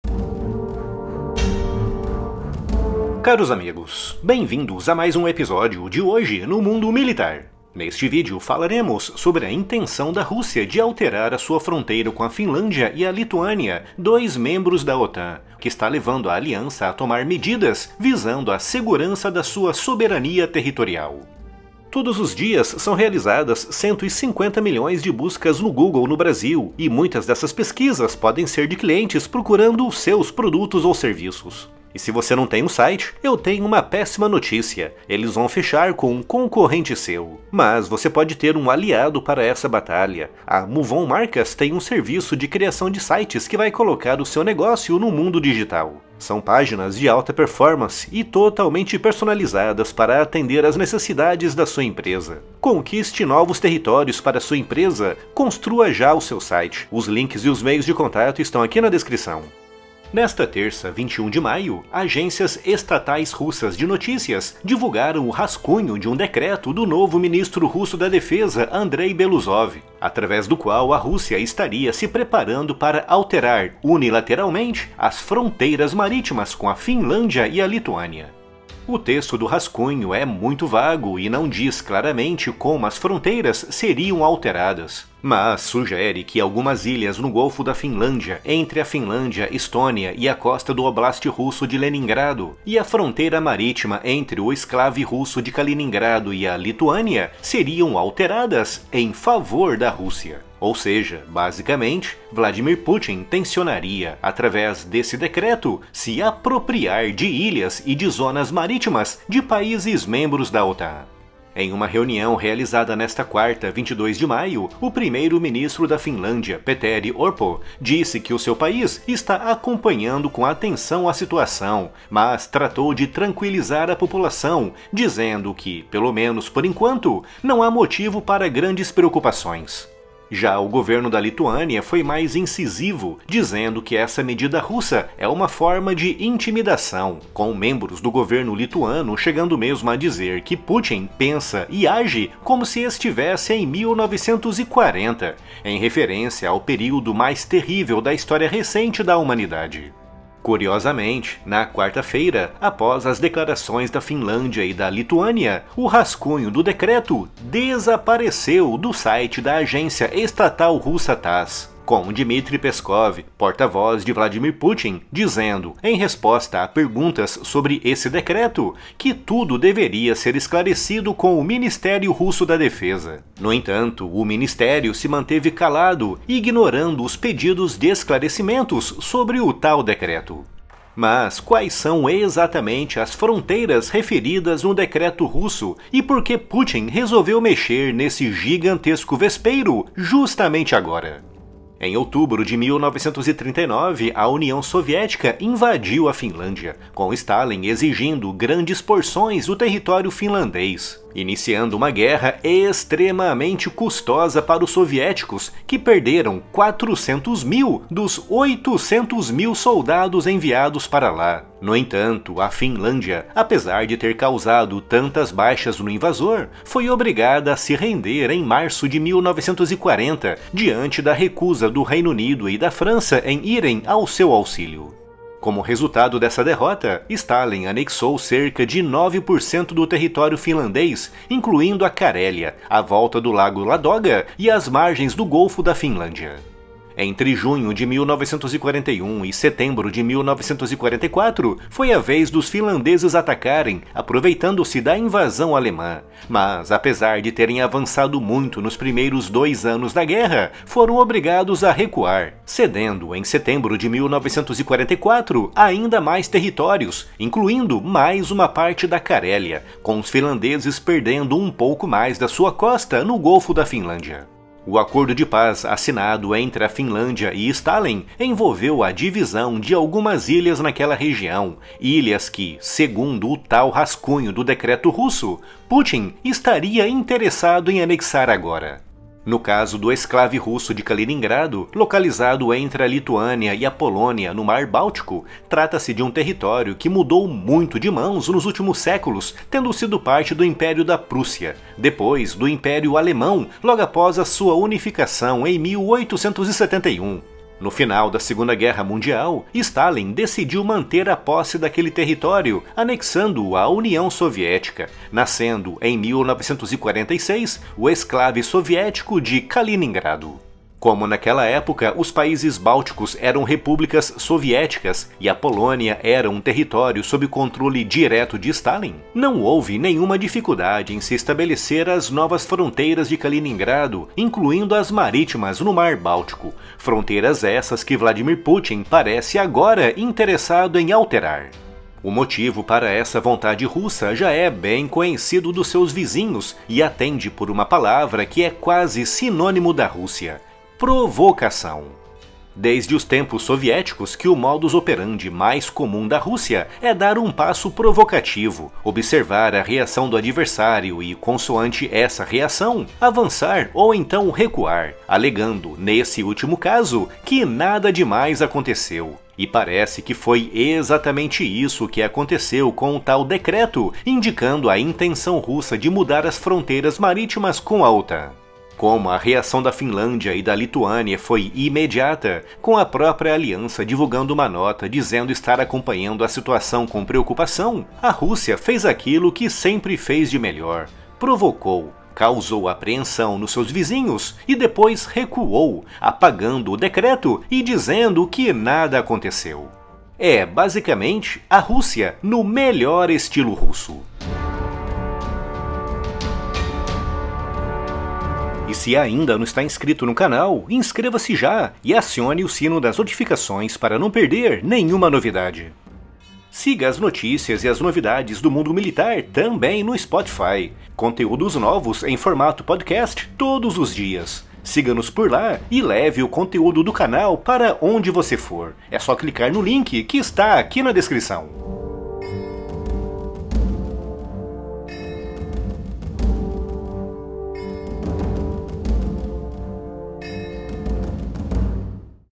Rússia quer alterar a sua fronteira com Finlândia e Lituânia – “Putin pensa que estamos em 1940!”
O site Hoje no Mundo Militar é a sua dose diária de informação militar e geopolítica.